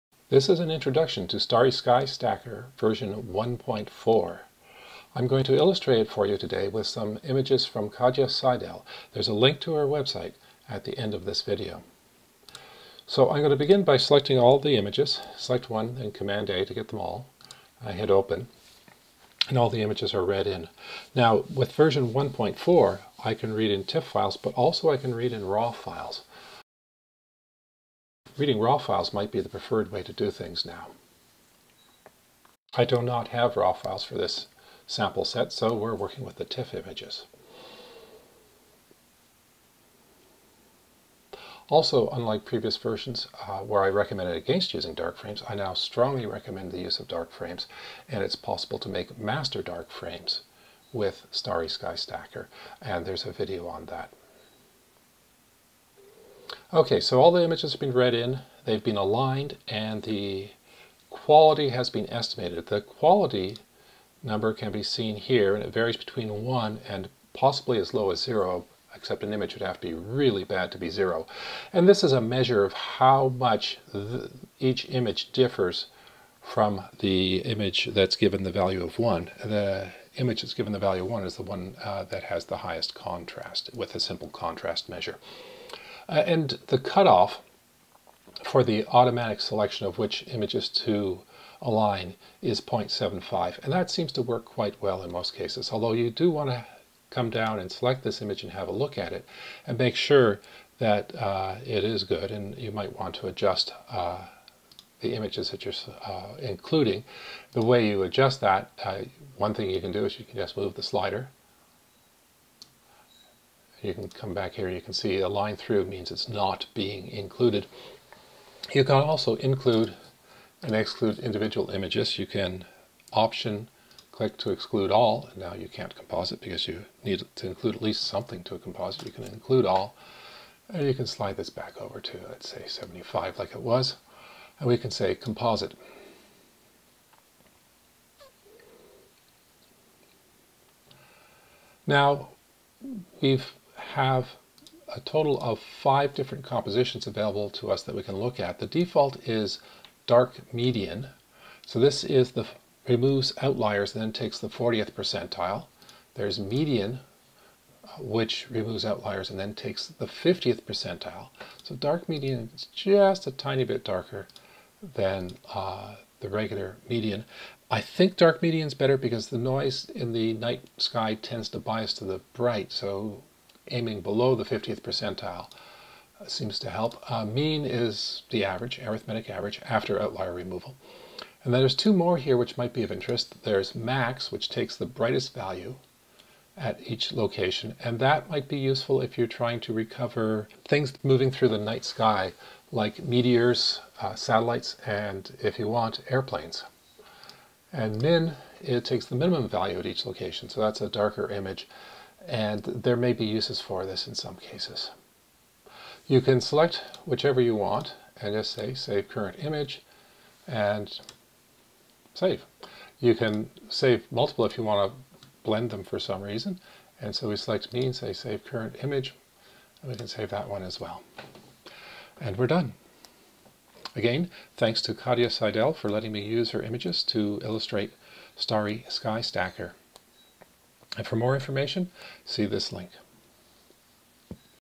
Starry Sky Stacker Version 1.4 Introduction
Tutorial introduction to Starry Sky Stacker Version 1.4. Starry Sky Stacker is an easy-to-use tool for aligning and stacking images of the night sky taken with a camera on an equatorial mount.